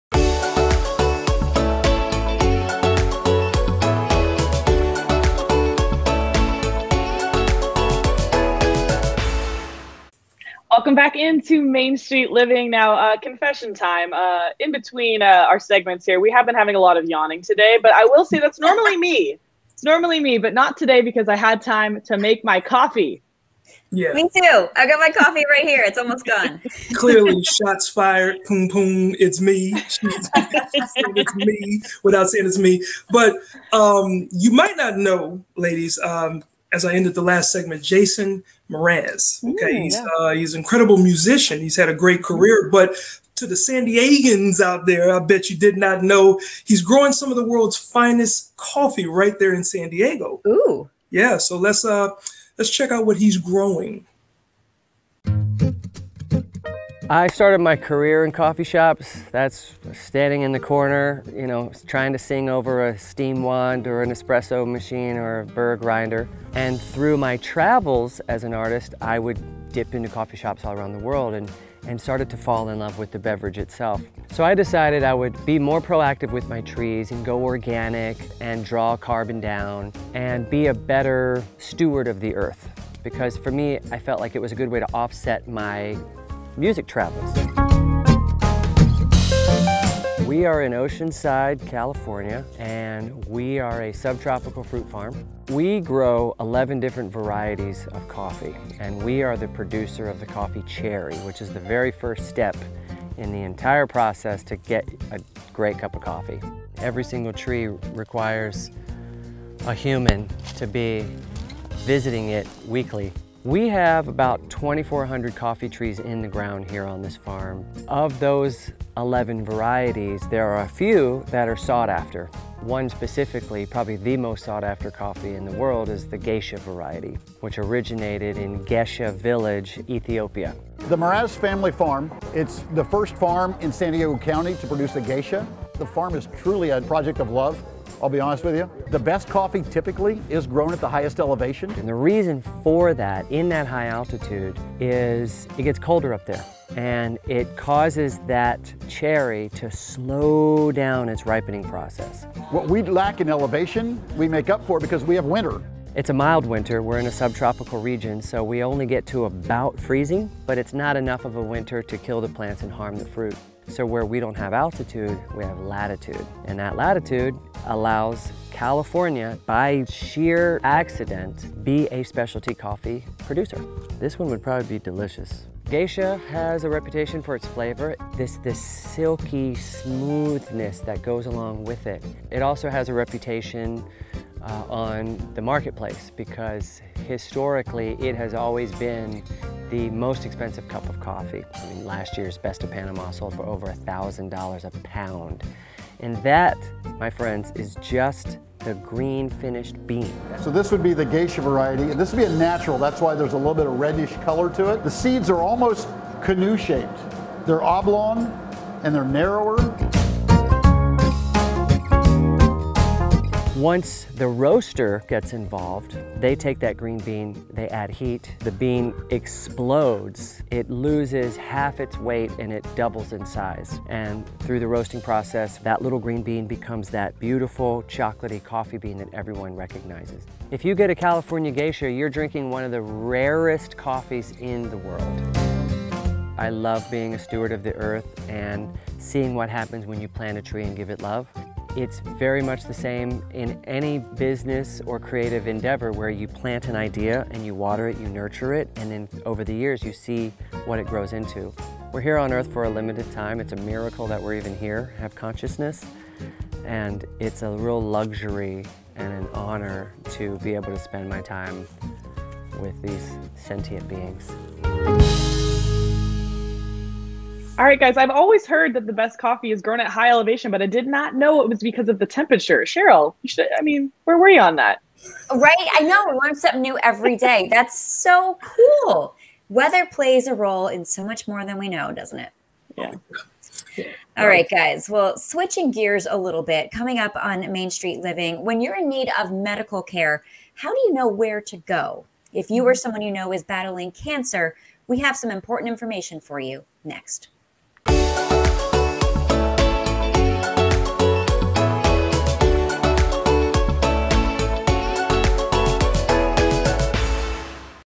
Coffee with Jason Mraz
You probably know Jason Mraz from his incredible music career.  (If not - he’s a two-time Grammy winner with an international fan base.)

Besides music, he’s also passionate about the environment and sustainability, and at Mraz Family Farms he’s growing the first California-grown Geisha coffee beans.

Jason planted his first coffee trees in 2015 on his farm in San Diego County.  He also grows avocados, passion fruit, dragon fruit and bananas at Mraz Family Farms – where their motto is to be organic, fair and kind.

You have to love that!